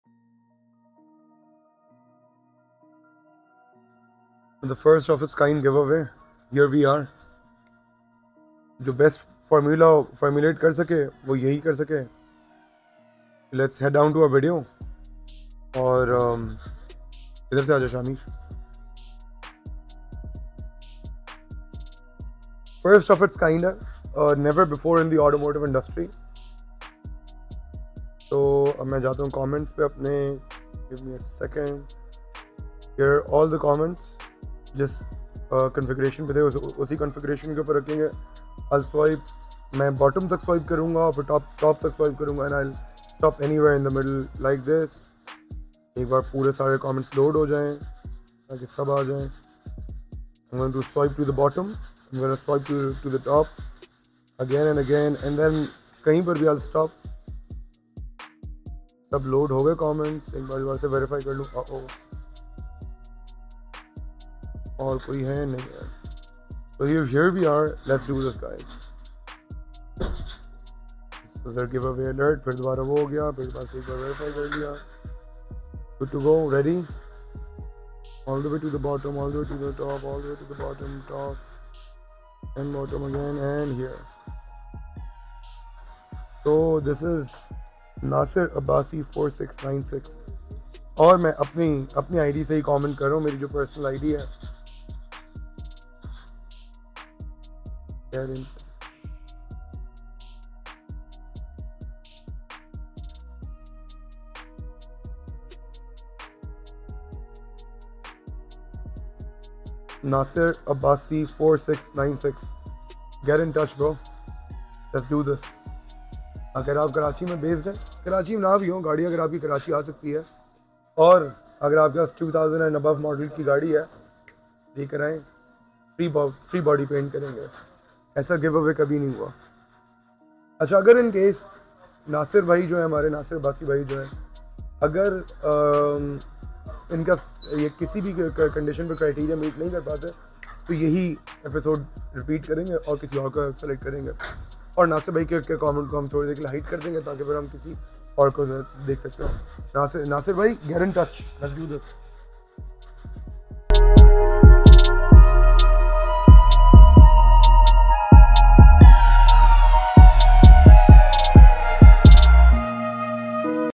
🎉 Congratulations to our Lucky Winner! 🎉 | Pakistan AutoTechnics
Join us in celebrating as we reveal the winner of our exciting giveaway!

PAT is the expert resolve to do this right.